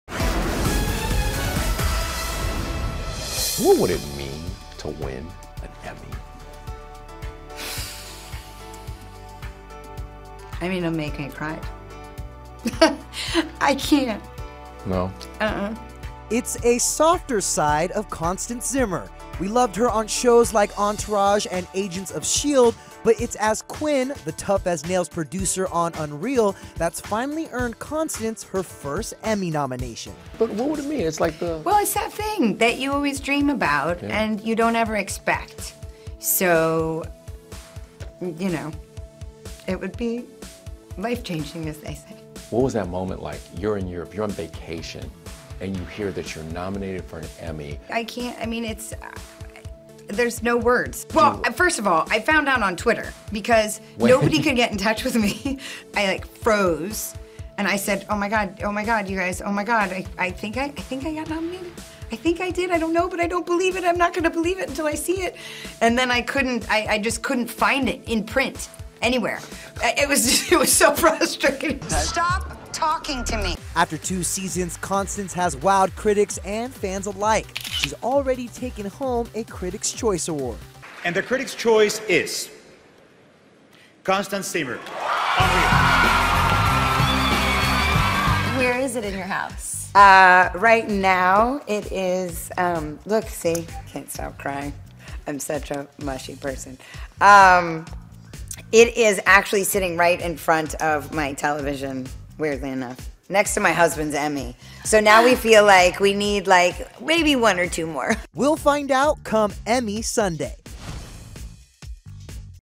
Why Constance Zimmer is So Emotional Over Her First Emmy Nomination for 'Unreal'
The actress opens up to ET about her first Emmy nomination.